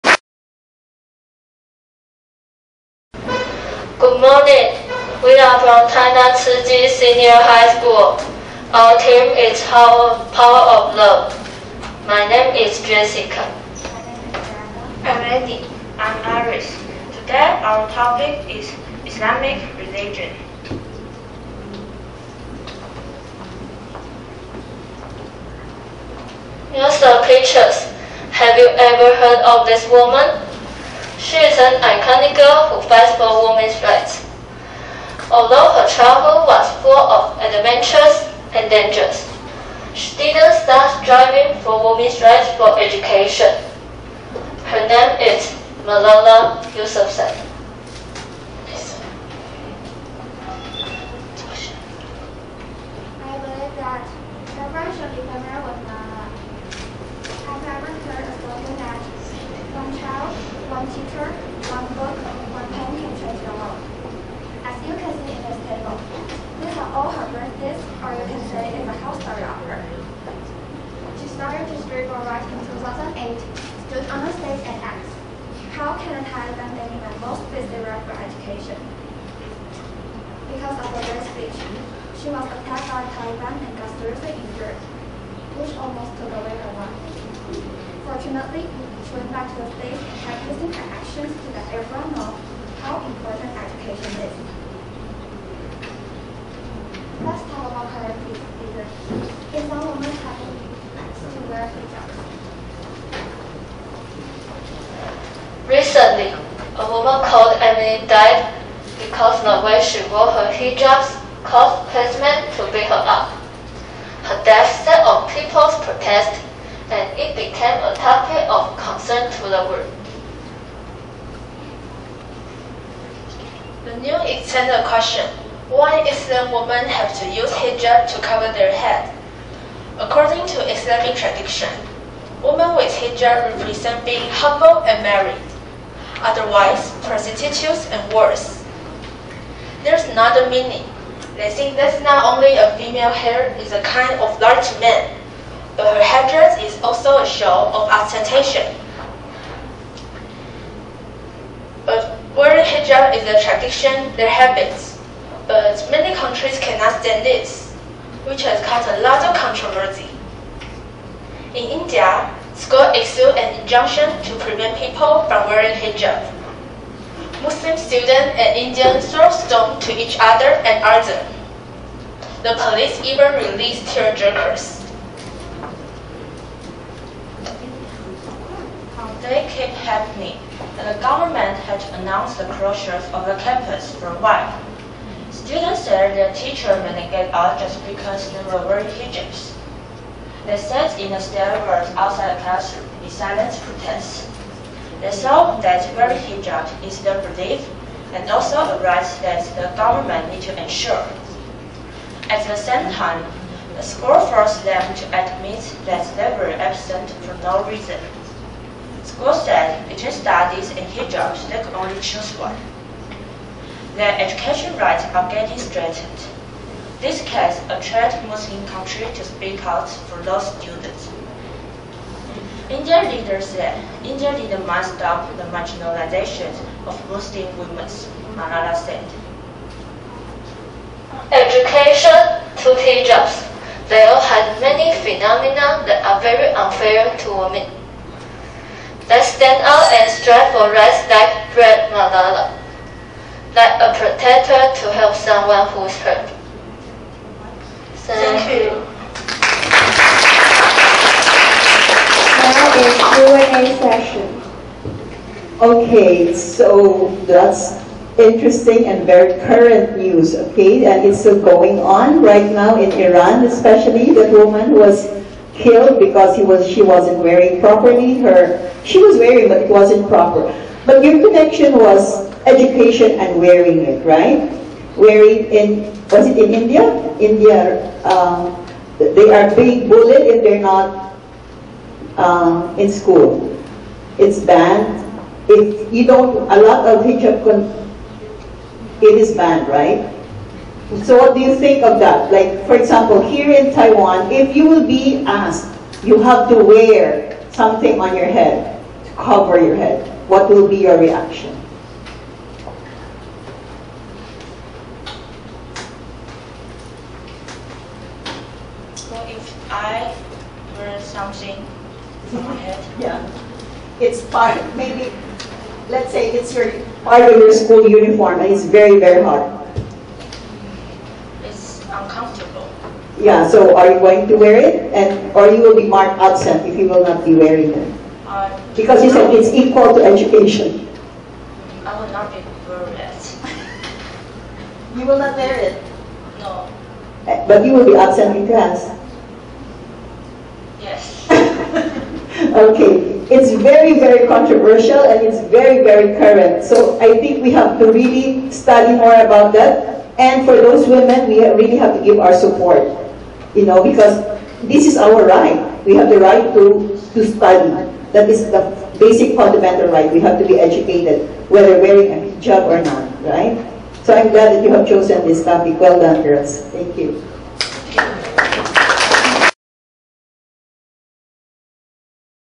G5 慈濟高中
School: 慈濟高中
Topic: Islamic religion
Participants: 王禹喬, 邱文琳, 郭尚真, 曾奕瑄